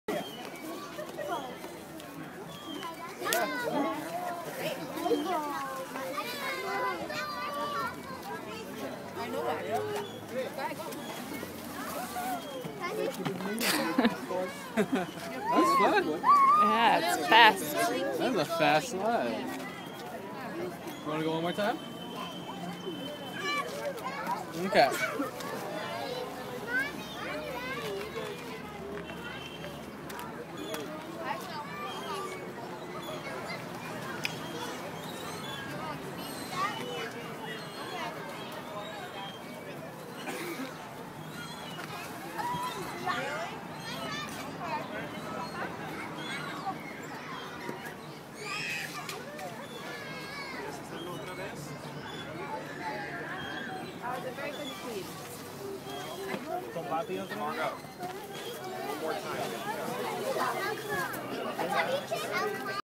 Central park slide
Lovely day today, biked to the Central Park zoo then discovered this great slide just north of the zoo. Miles did great for both legs of the trip. I see many future rides on the Chariot in our future.